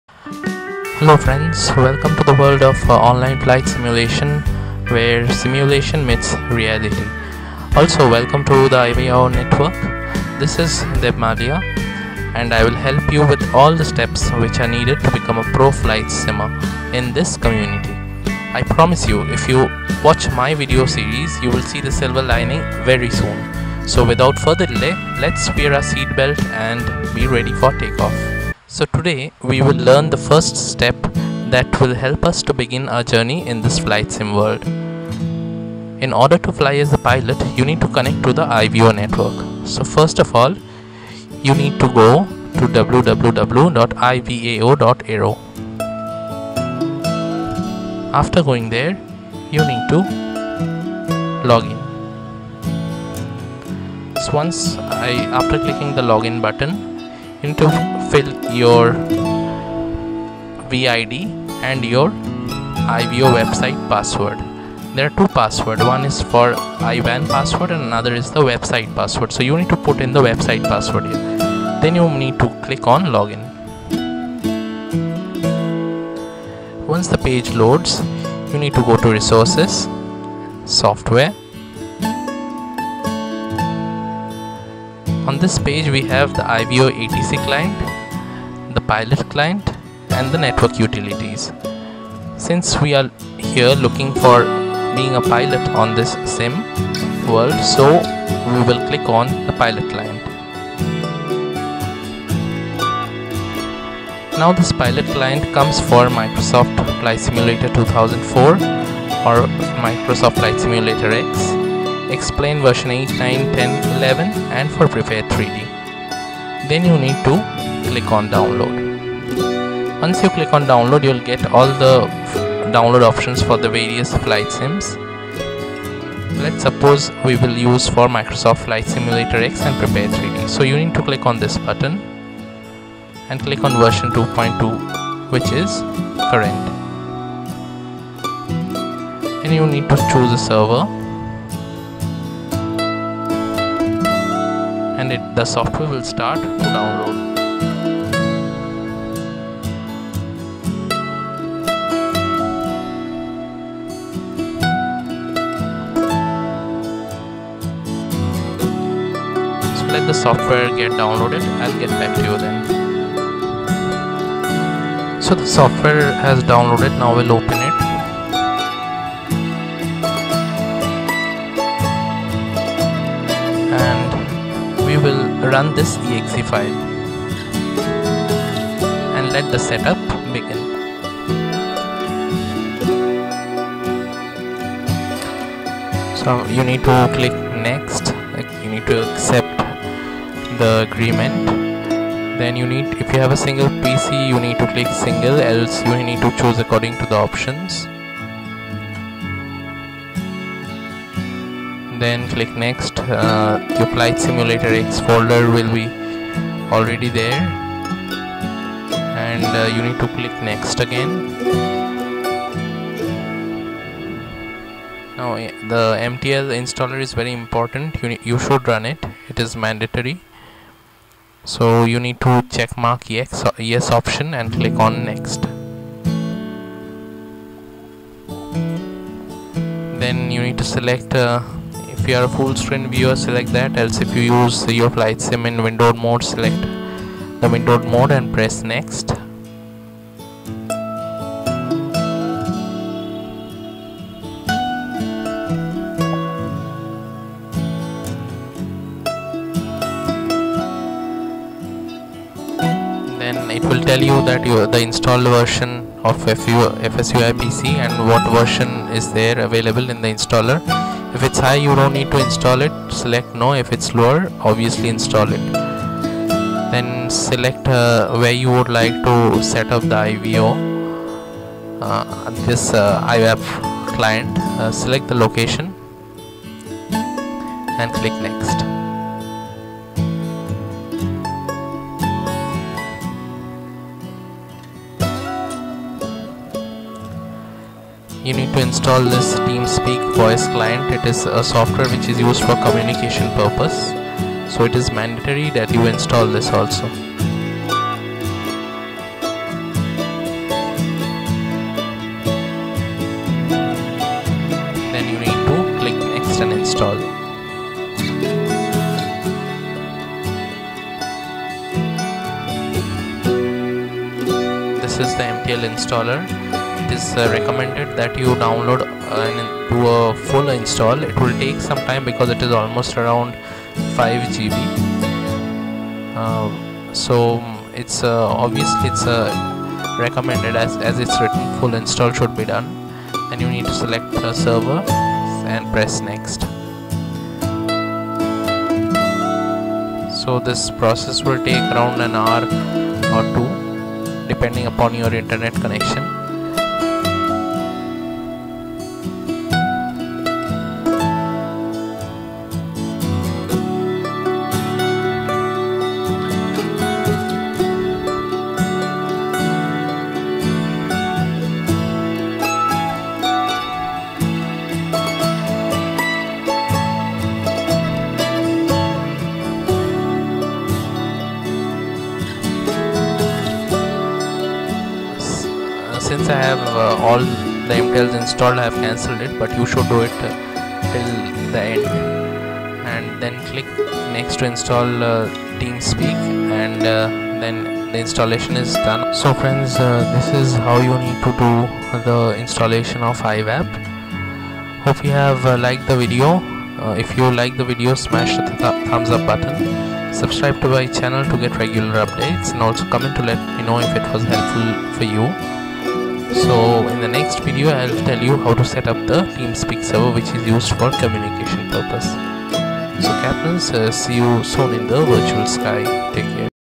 How to set up IVAP: Beginners Guide to IVAO
In this video i will show you how to install IVAP client on you system so that you can start your online Virtual Flying Career on IVAO.

If you like the video SMASH the thumbs up button, comment and share. See you soon in the virtual sky.